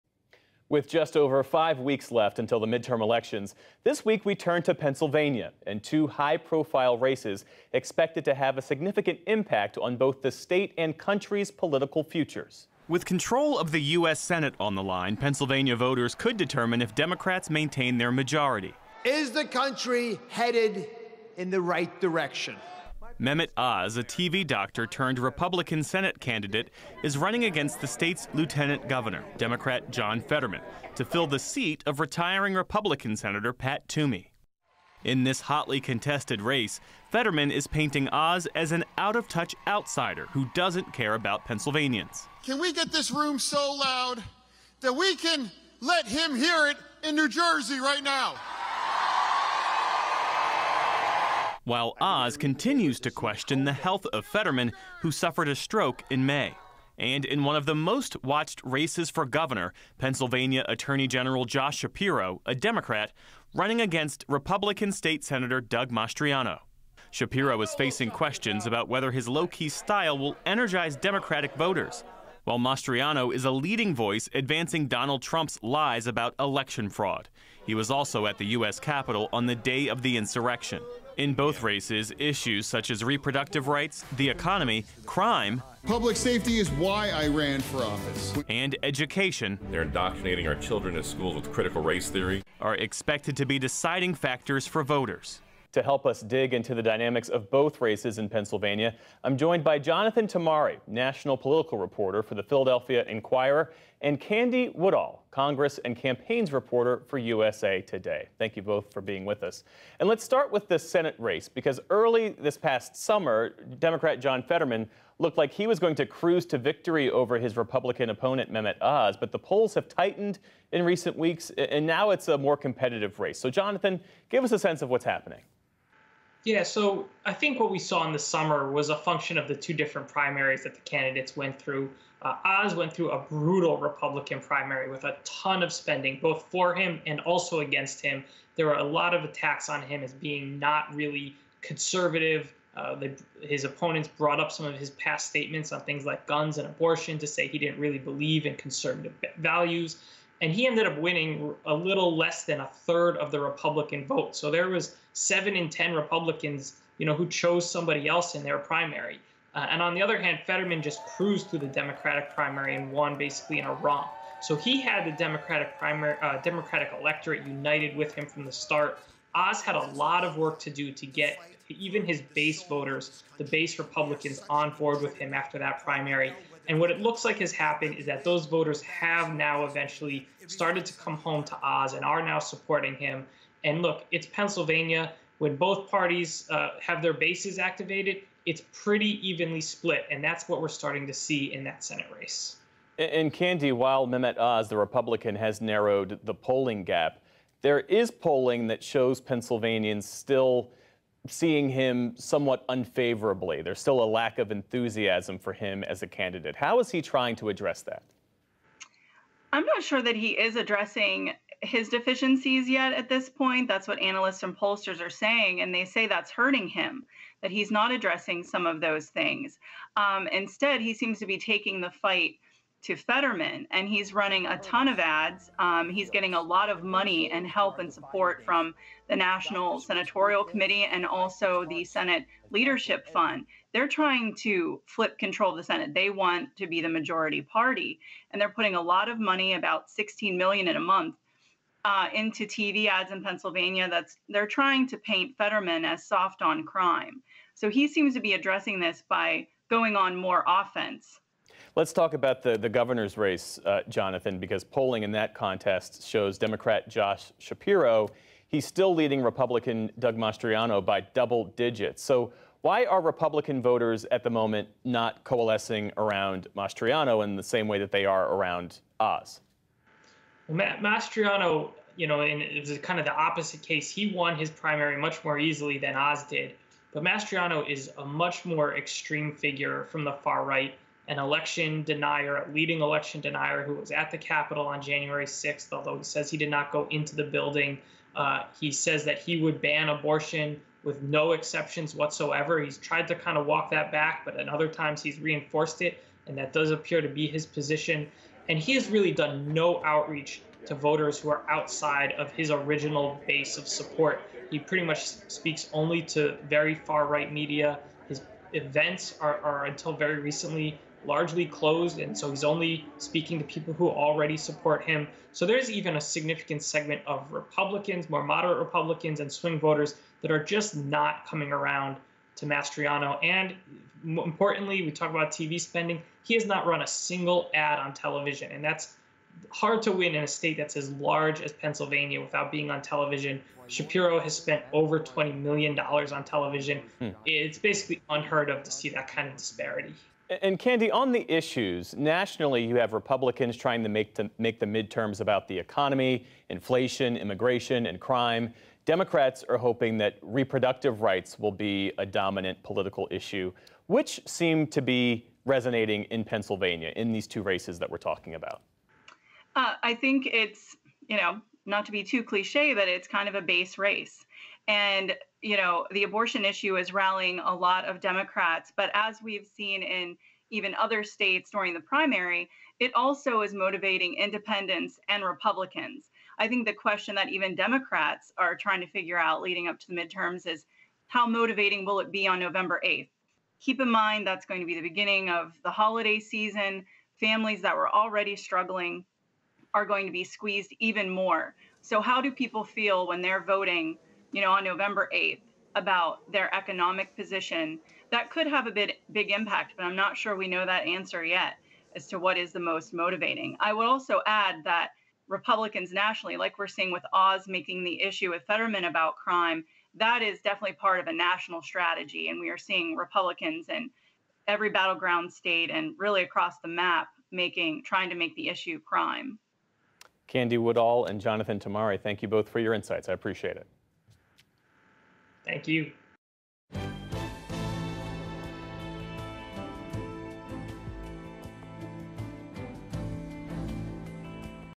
How Pennsylvania’s midterm races impact the future of politics
With just over five weeks left until the midterm elections, we look at Pennsylvania and two high-profile races expected to have a significant impact on both the state and country's political futures. Jonathan Tamari, national political reporter for the Philadelphia Inquirer, and Candy Woodall, Congress and campaigns reporter for USA Today, join Geoff Bennett to dig into the dynamics of both races.